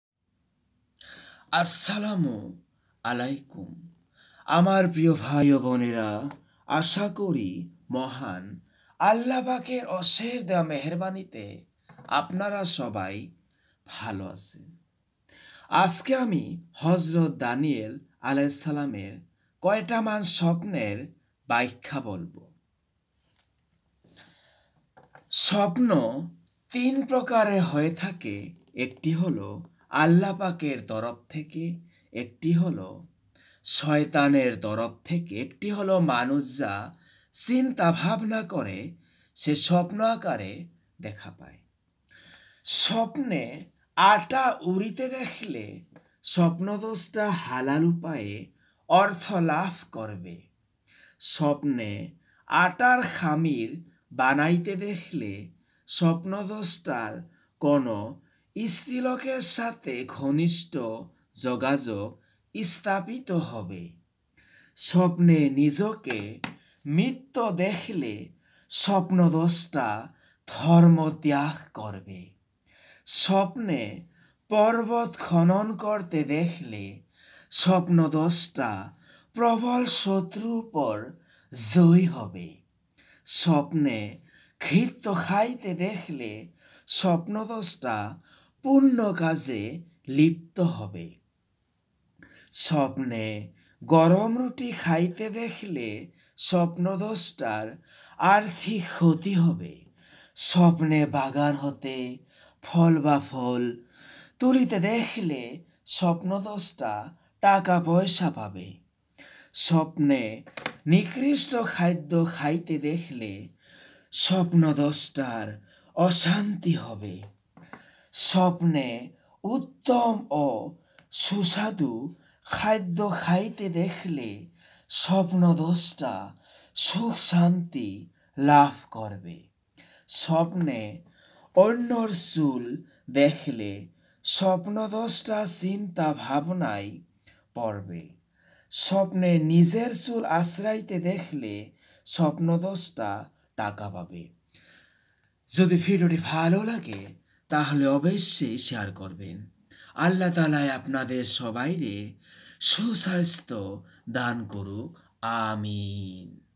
স্বপ্নে ঘী খাইতে দেখলে কি হয়।হজরত দানিয়েল (আ:)এর কয়েকটি স্বপ্নের বাখ‍্যা।( part : 1)
এই video টিতে হজরত দানিয়েল (আ:)এর কয়েকটি মান স্বপ্নের বাখ‍্যা বলা হয়েছে।